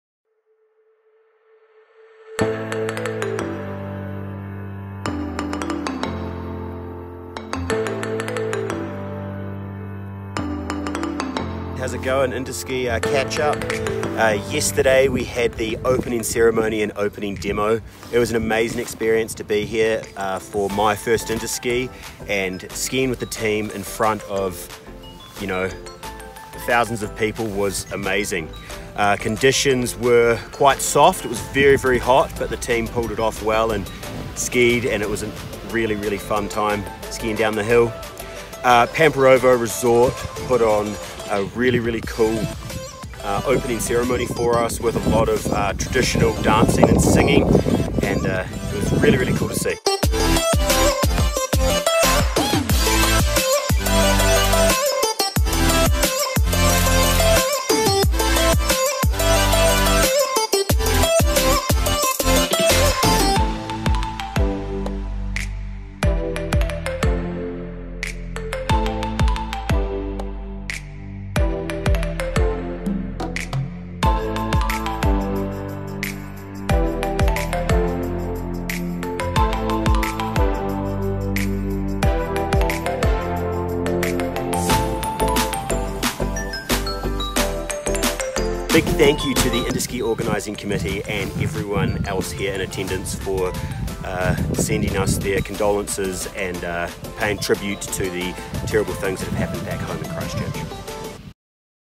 Interski Day 1
A quick update from team member Harry McFadden on what the team has been up to at Interski in Pamporovo, Bulgaria